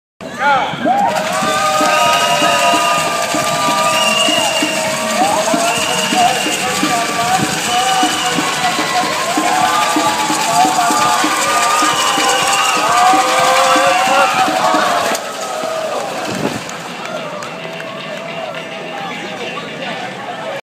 25th anniversary of a Leicester HIV and Aids charity
Boat race in Leicester city centre to mark 25th anniversary of a Leicester HIV and Aids charity